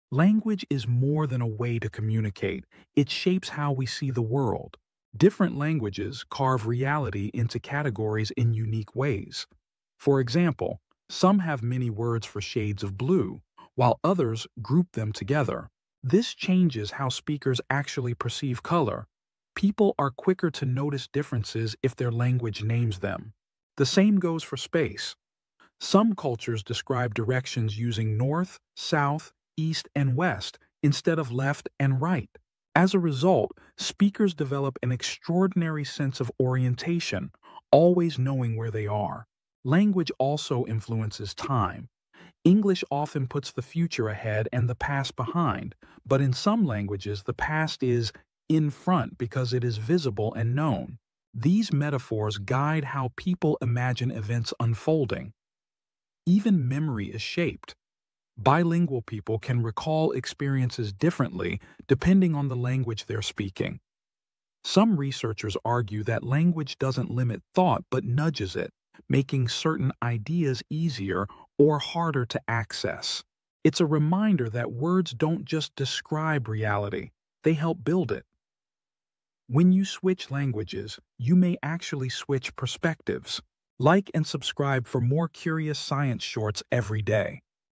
How Does Language Change the Way We Think? 🗣️🧠
Language doesn’t just describe the world—it shapes perception, memory, and even the way we imagine space and time. 🗣️🧠